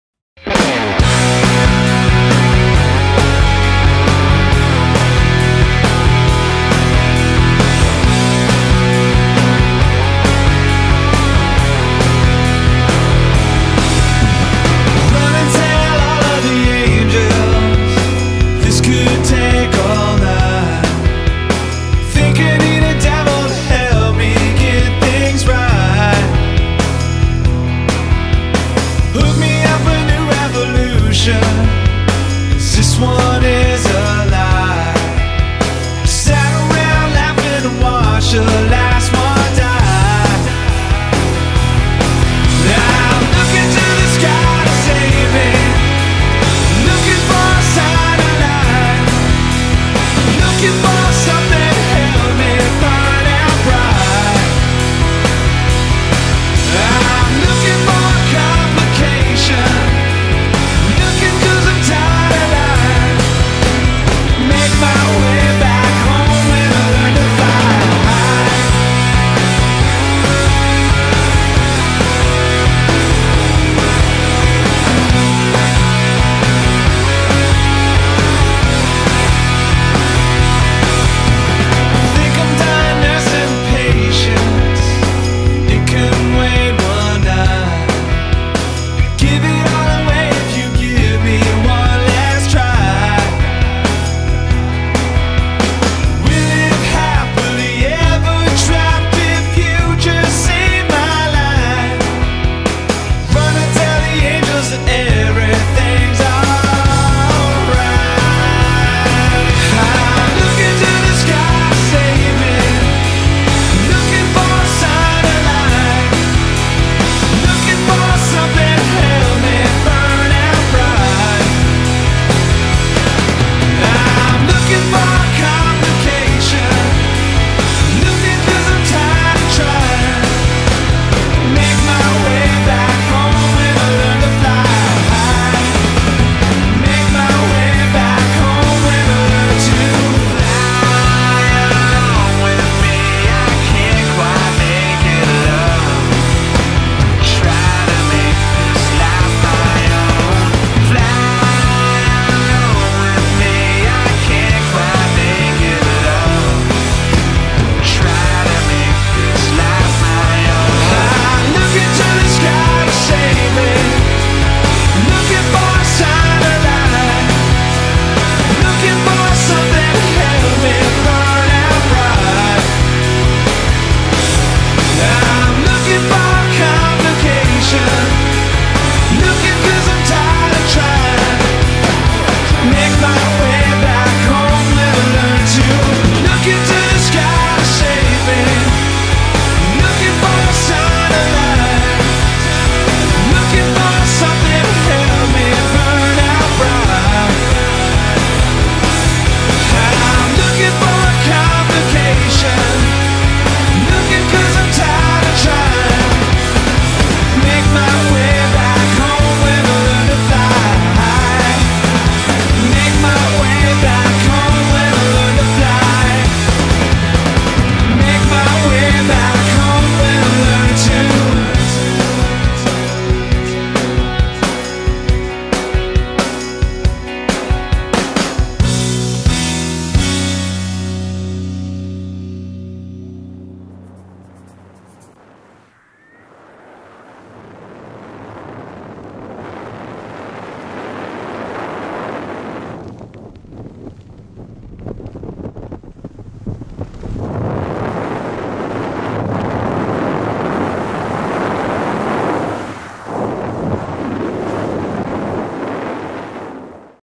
Paul Finney's Tandem Skydive
Paul Finney's Tandem Skydive at Skydive Tilstock,Whitchurch.